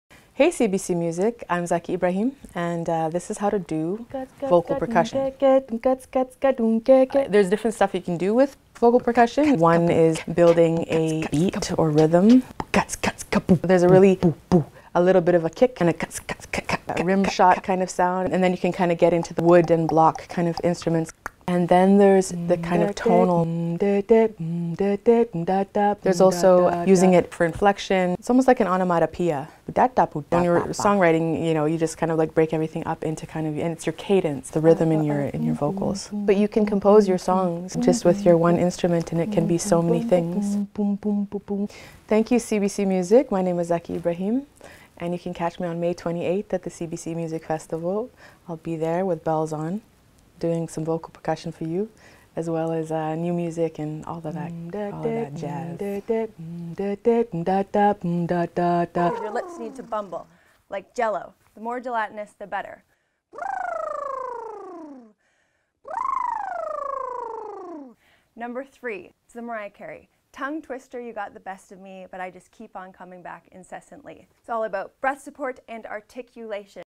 How-to Use Vocal Percussion With Zaki Ibrahim
Zaki Ibrahim teaches us how to use vocal percussion when writing songs.